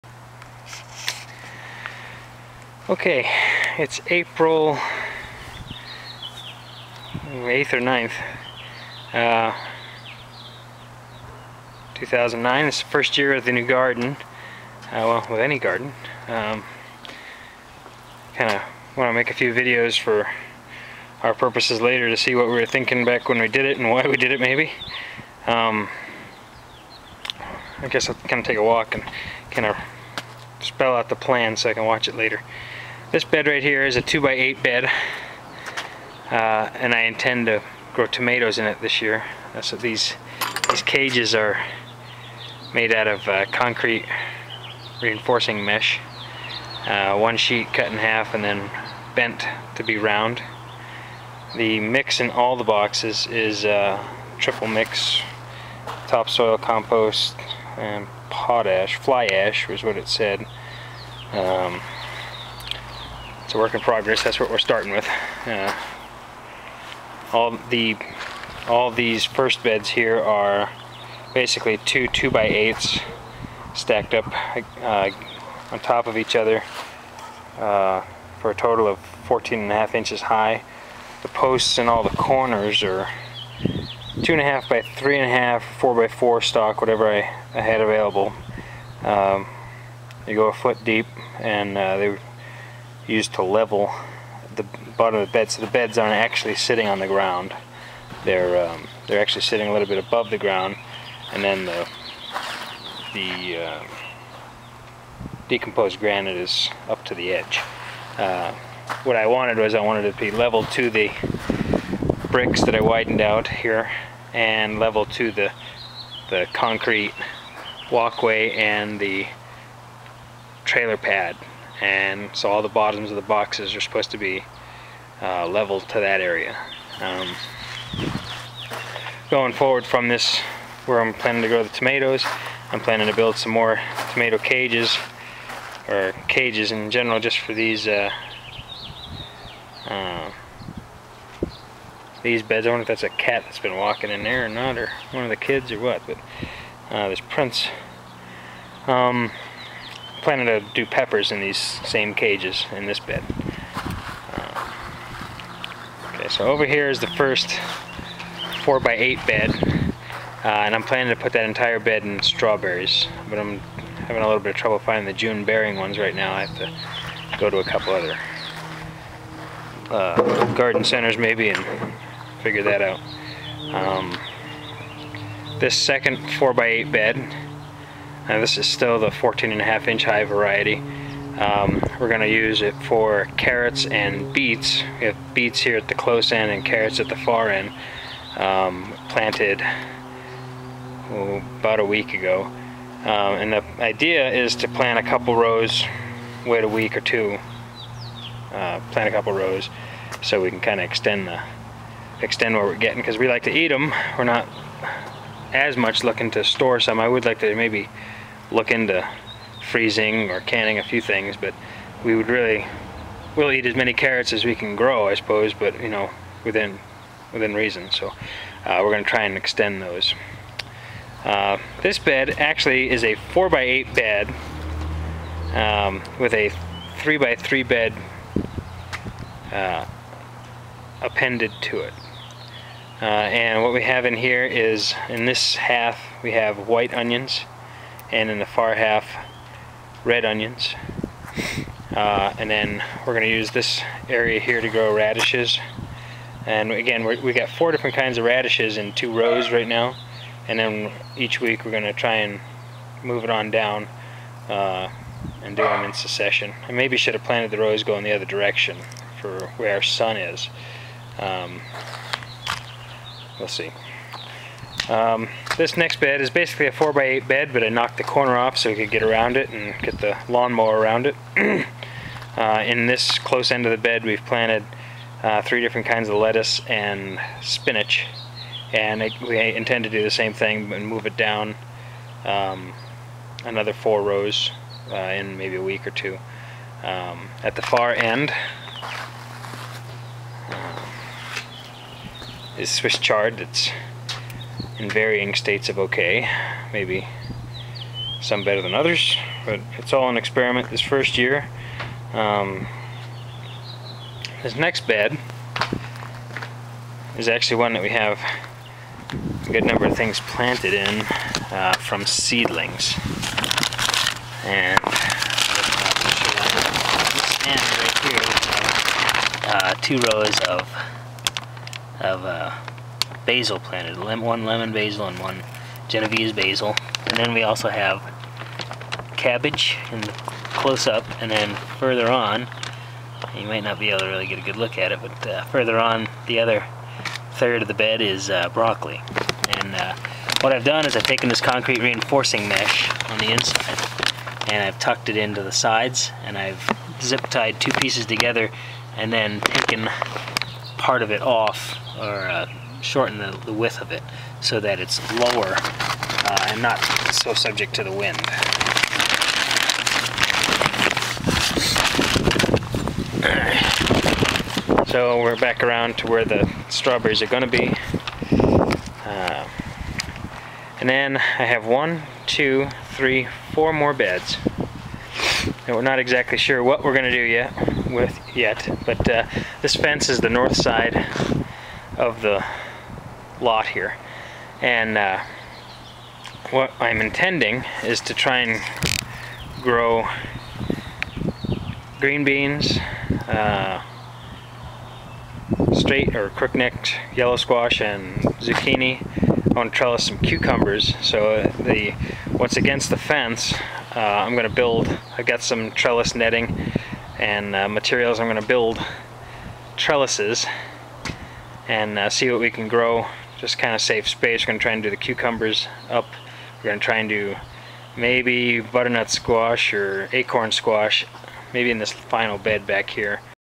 Rob's Raised Beds April 2009
Initial video of raised garden beds.   April 2009.